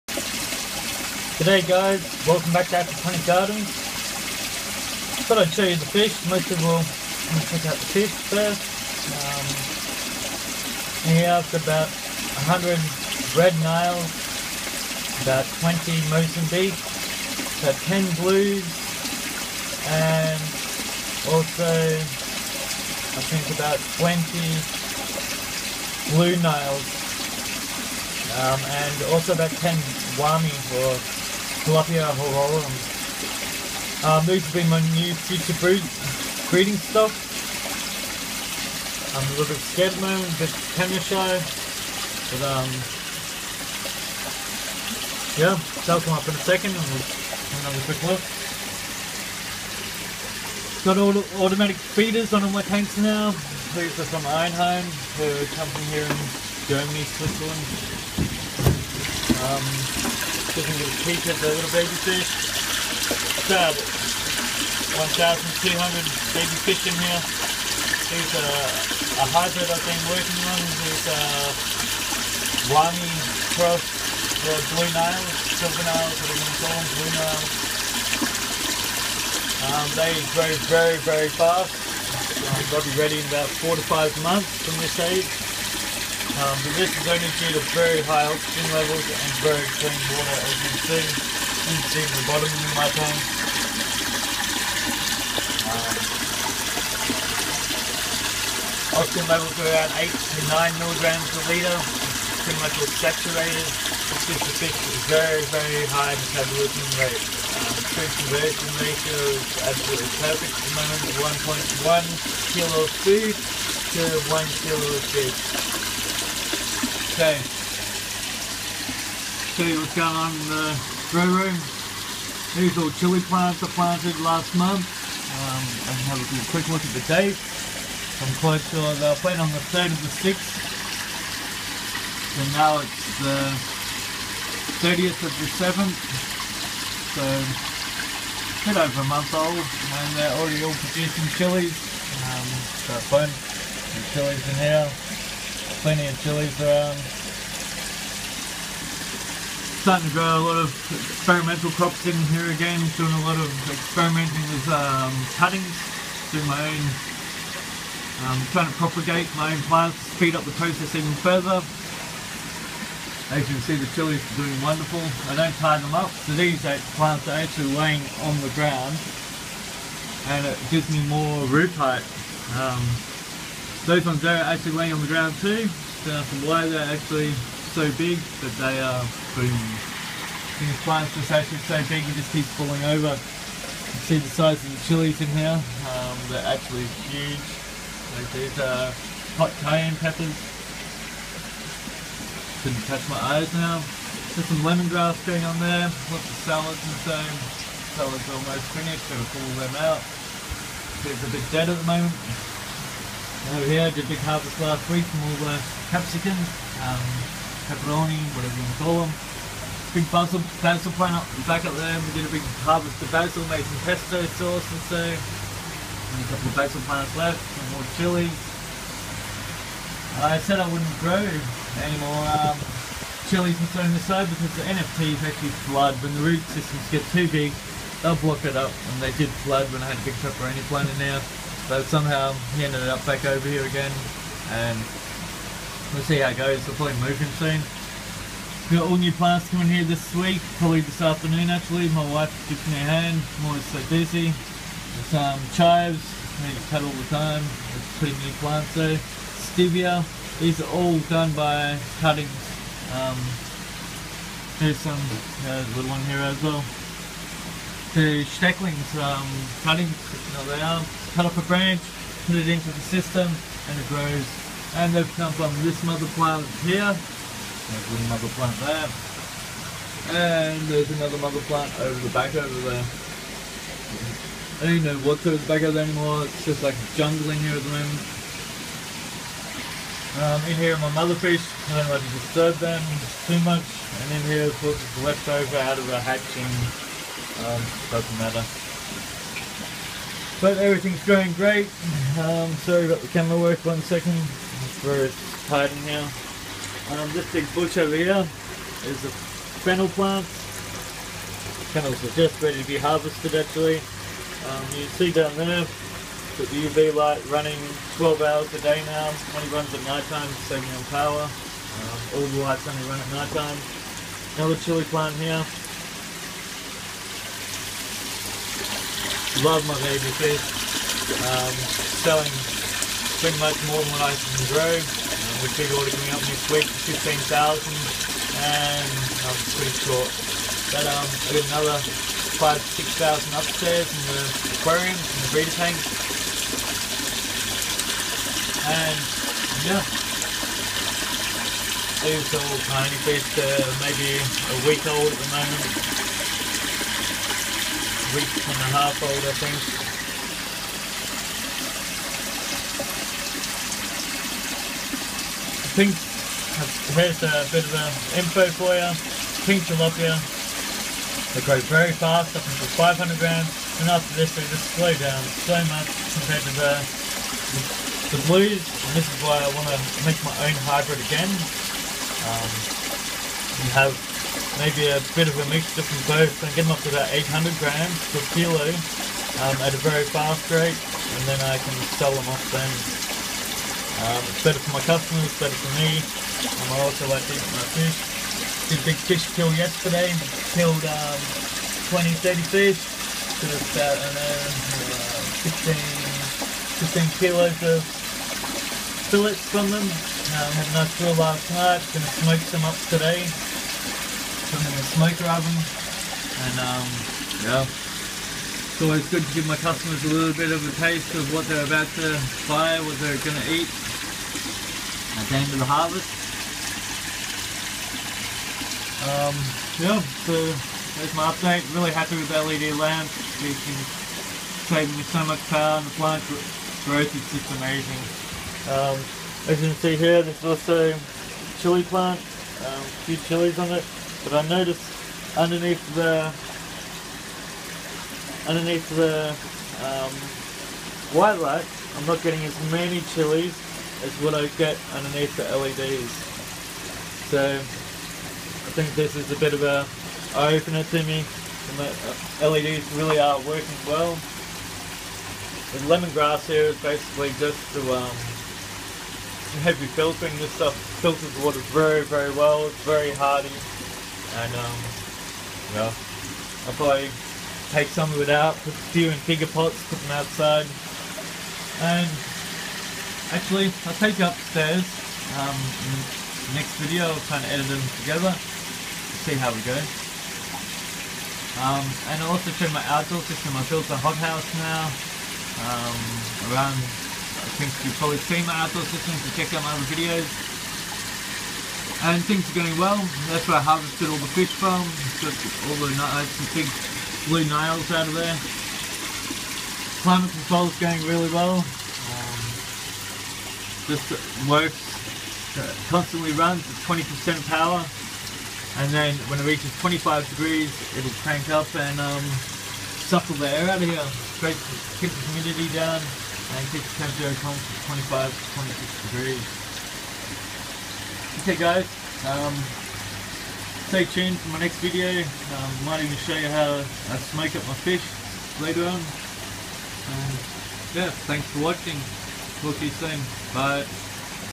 Tilapia Farm with the use of Aquaponics (update)
Hey everyone... thought I would show a update on how the Tilapia breeding farm is going... Aquaponics makes life interesting and problem free when it comes to fish farming.. I have had no fish kills in 16 years of Aquaponics due to the fact that I integrate Aquaponics in all my systems..Plant roots and material is used in a lot of organic medicaments and even antibiotics.. make sure to include as much variety of plants as possible..Garlic, chives, lavender, chilli are some good examples of plants used..